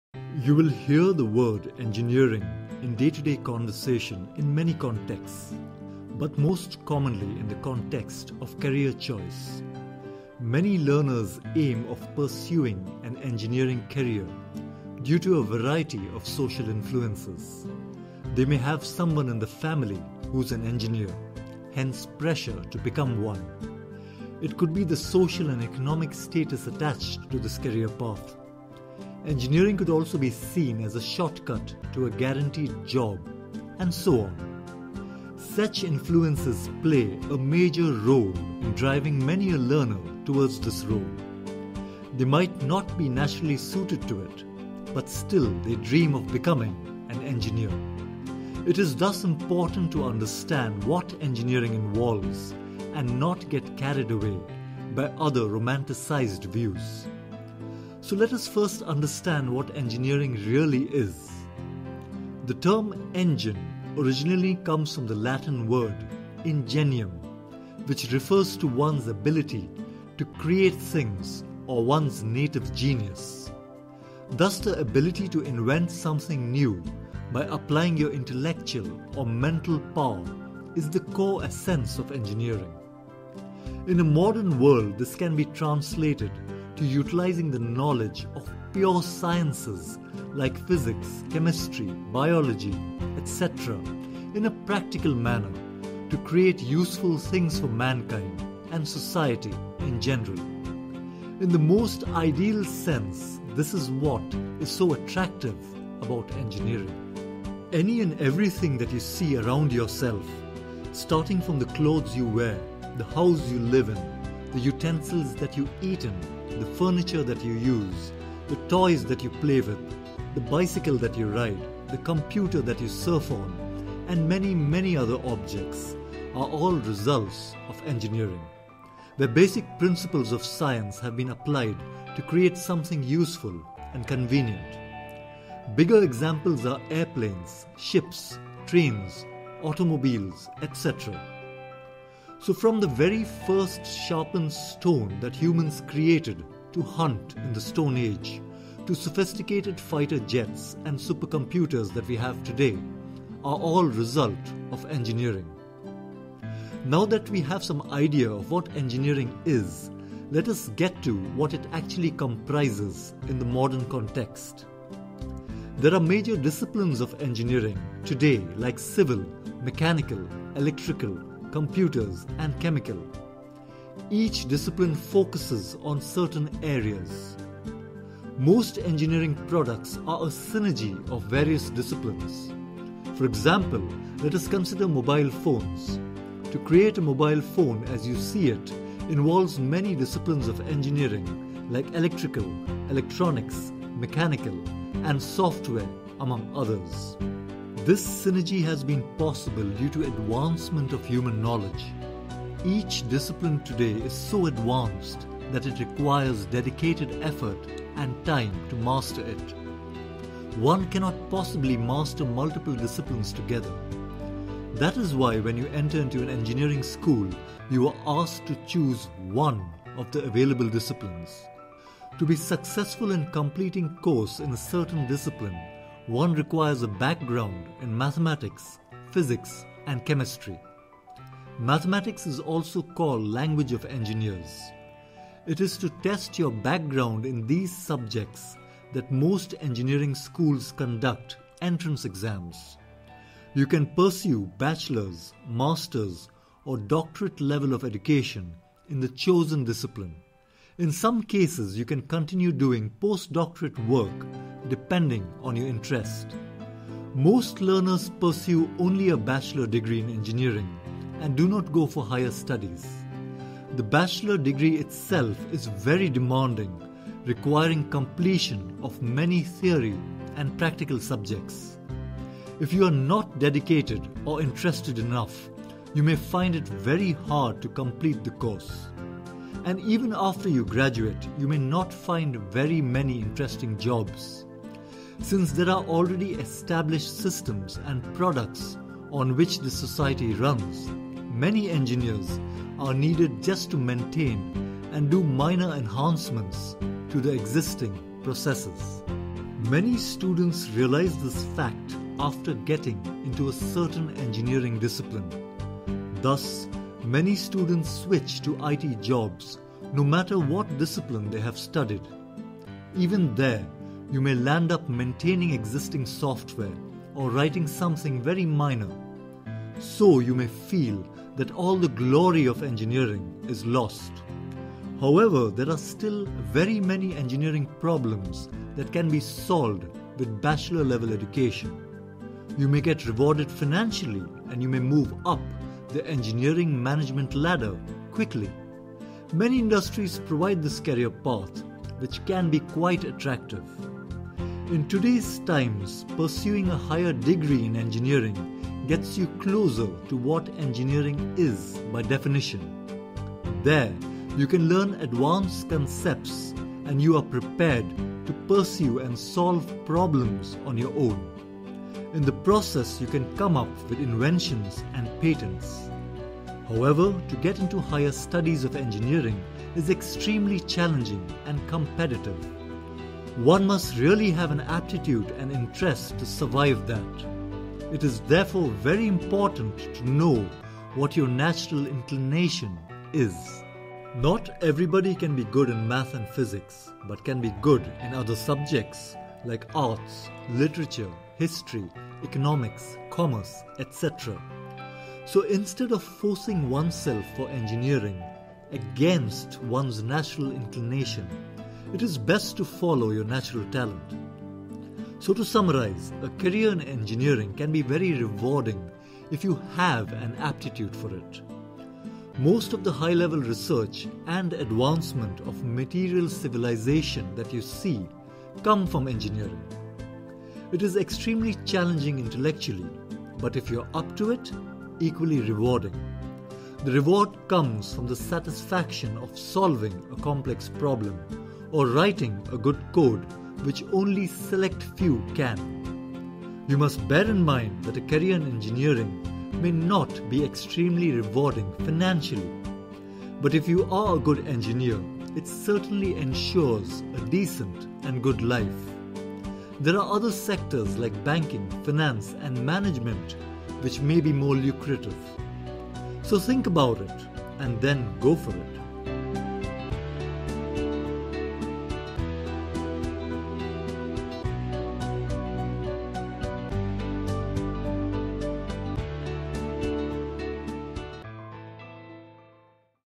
Engineering, an overview. Careerbuilder Videos from funza Academy
The term engineer originally comes from the Latin word 'ingenium' which refers to one's ability to create things or one's native genius. This video will give you a high level overview about what engineering as a career choice is all about.
This site also has videos on various educational concepts like The Water Cycle Etc.

We at funza have interviewed professionals regarded as experts in their domain and come up with
this video. It focuses on providing answers to these questions:

a. Overview of the role
b. Requirements of the role
c. Joys of the role
d. Challenges of the role
e. An average day
f. Future prospects